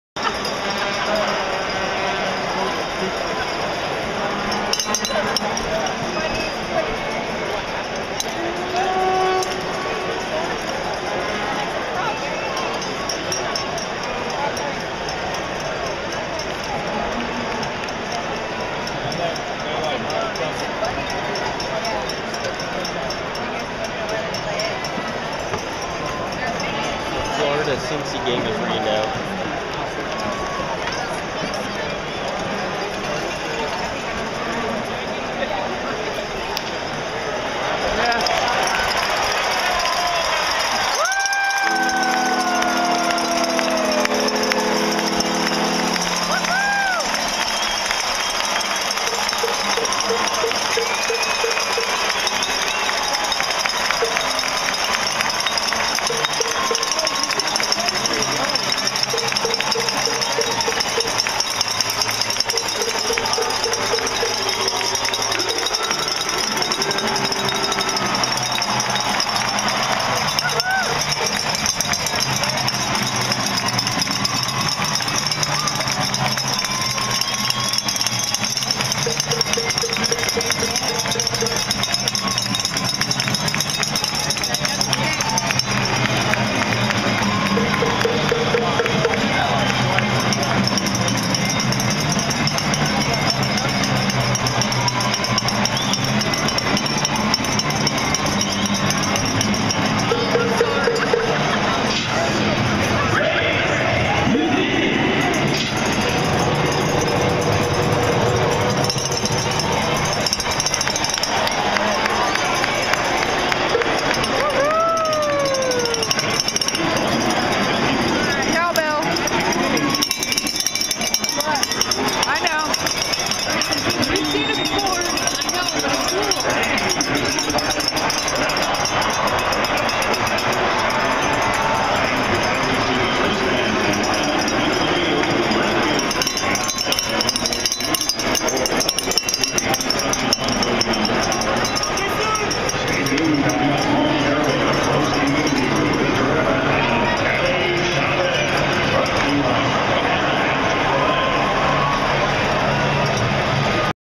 Rays Win vs. Baltimore 8/14/2010
The final out for the Rays vs. the Baltimore Orioles. They won 7-3. Kelly Shoppach was the hero with a Grand Slam and solo HR.